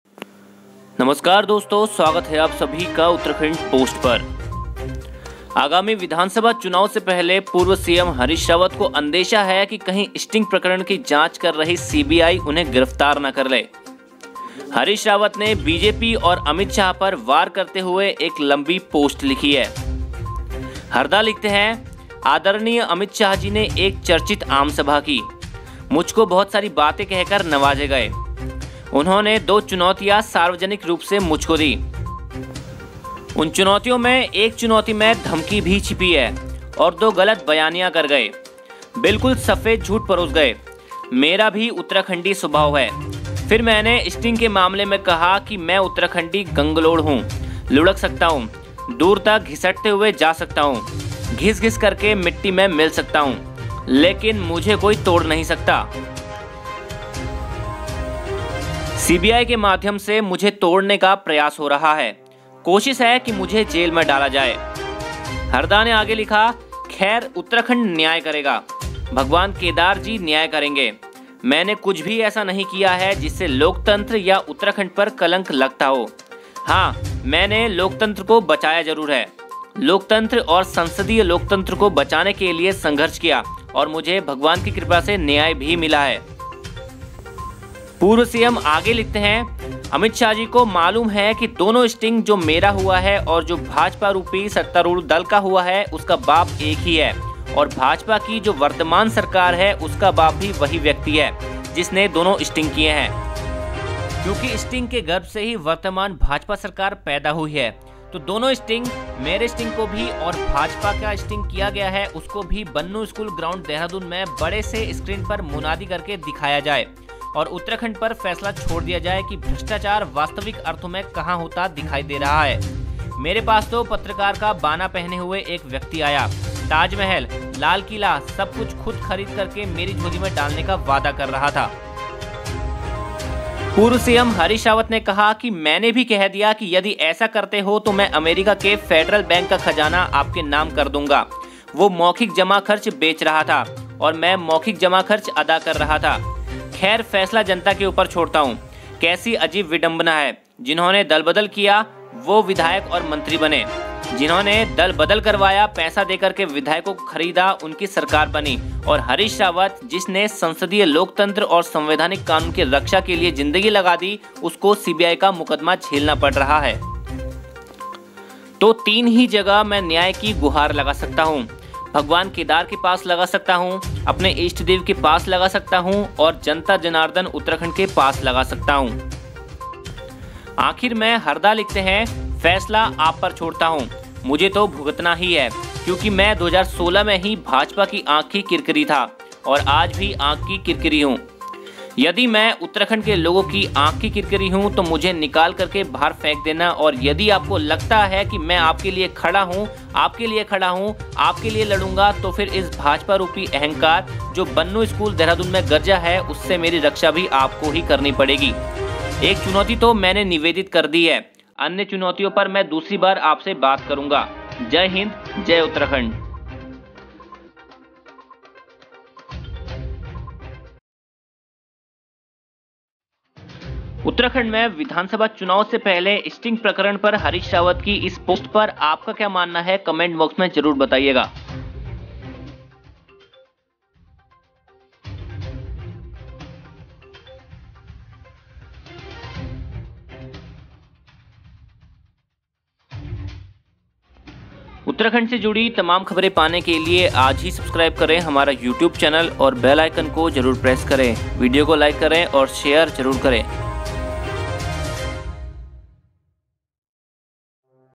उत्तराखंड विधानसभा चुनाव से पहले हरीश रावत को गिरफ्तार करेगी CBI ! | Uttarakhand Harish Rawat Sting
आगामी विधानसभा चुनाव से पहले पूर्व सीएम हरीश रावत को अंदेशा है कि कहीं स्टिंग प्रकरण की जांच कर रही सीबीआई उन्हें गिरफ्तार न कर ले। हरीश रावत ने बीजेपी और अमित शाह पर वार करते हुए एक लंबी पोस्ट लिखी है। क्या है पूरा माजरा, वीडियो में जानिए-